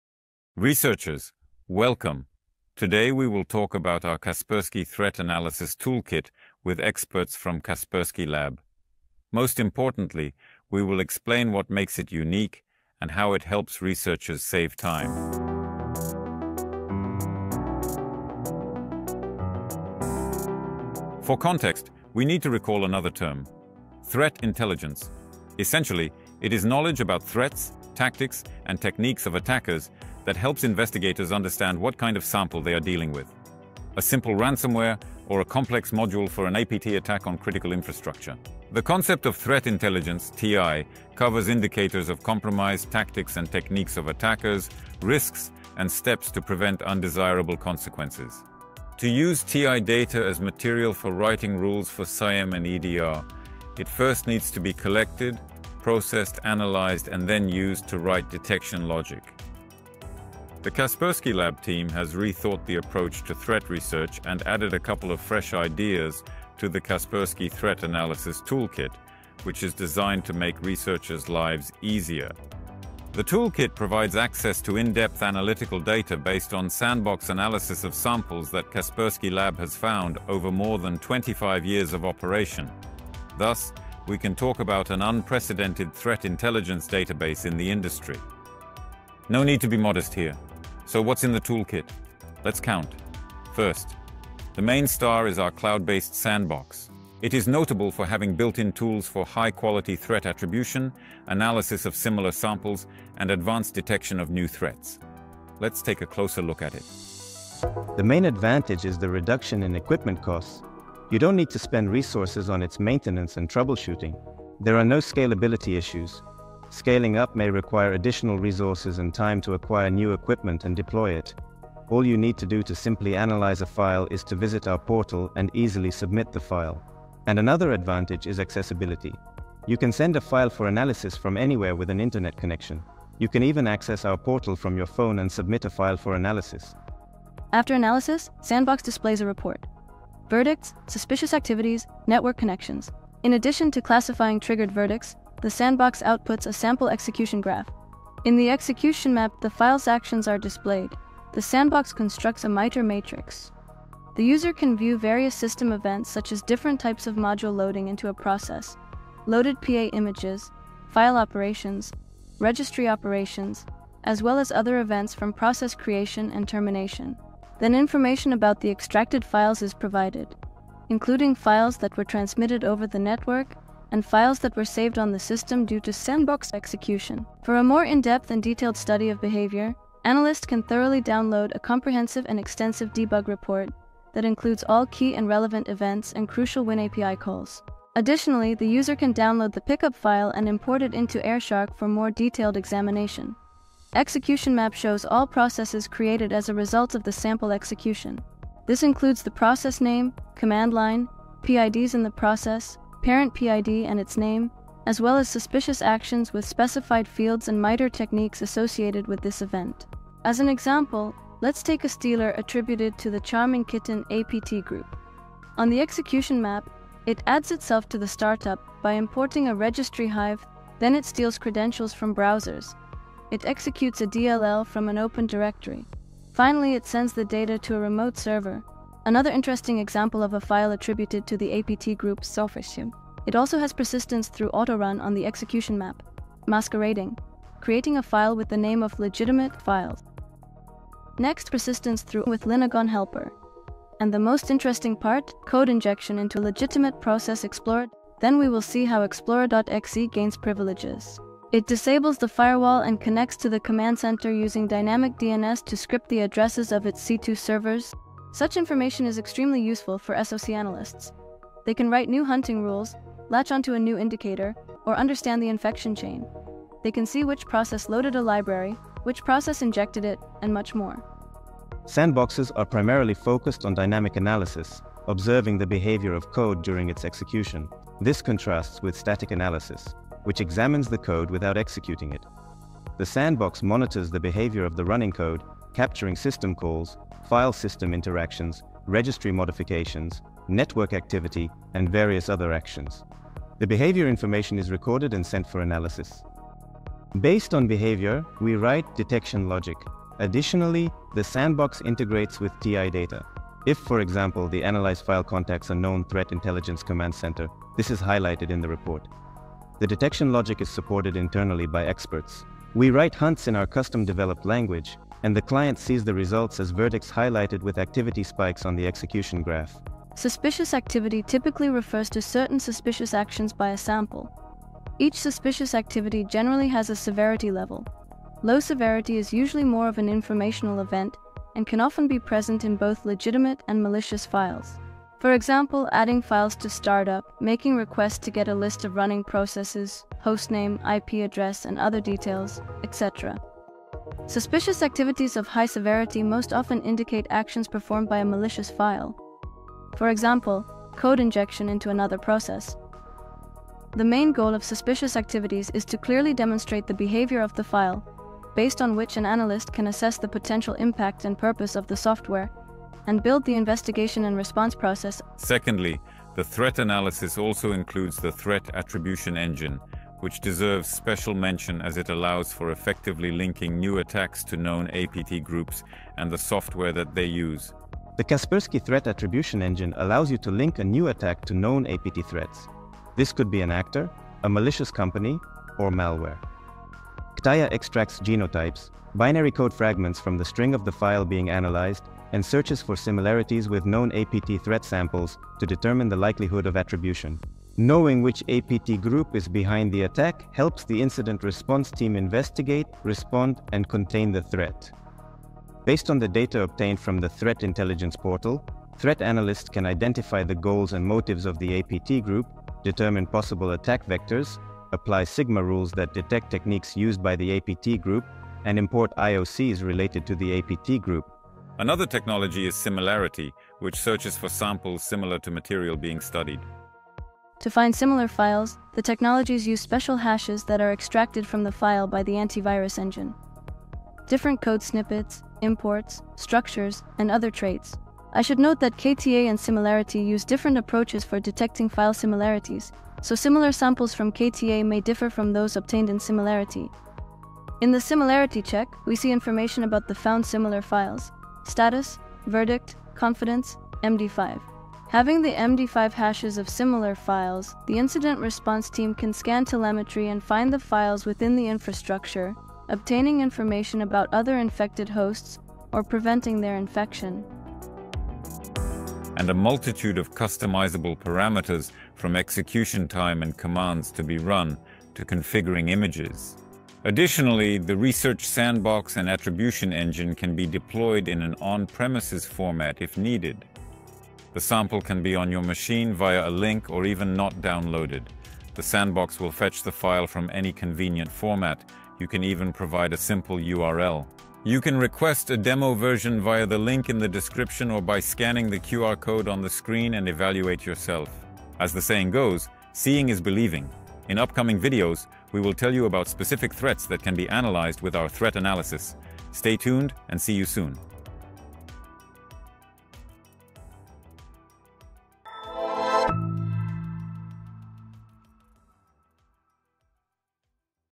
Unlocking Advanced Threat Analysis with Kaspersky Cloud Sandbox
Join us in today's video as we dive deep into the world of Threat Analysis with our experts. Discover what makes our tools unique and how they help researchers save valuable time. We’ll start by understanding Threat Intelligence – the critical knowledge of threats, tactics, and techniques used by malicious actors.

Our main component, the Kaspersky Cloud Sandbox, features built-in tools for quality threat attribution, analysis of similar samples, and advanced detection of new threats. Learn about the comprehensive nature of Threat Intelligence, from indicators of compromise and tactics to specific recommendations for avoiding undesirable outcomes. We'll also discuss how to use TI data for writing rules for SIEM and EDR by collecting, processing, and analyzing it to develop effective detecting logic.

Gain access to in-depth analytical data based on over 25 years of sandbox analysis.